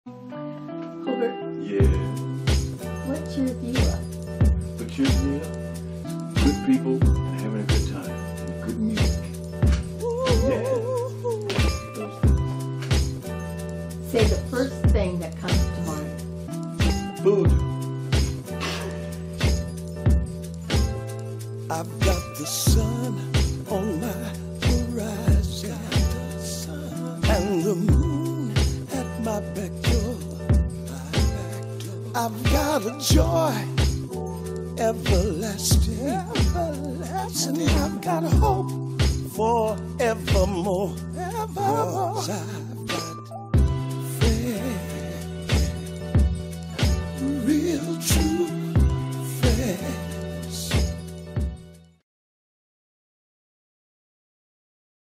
friends
Hubert Tubbs Video Meme - talking about what cheers him up.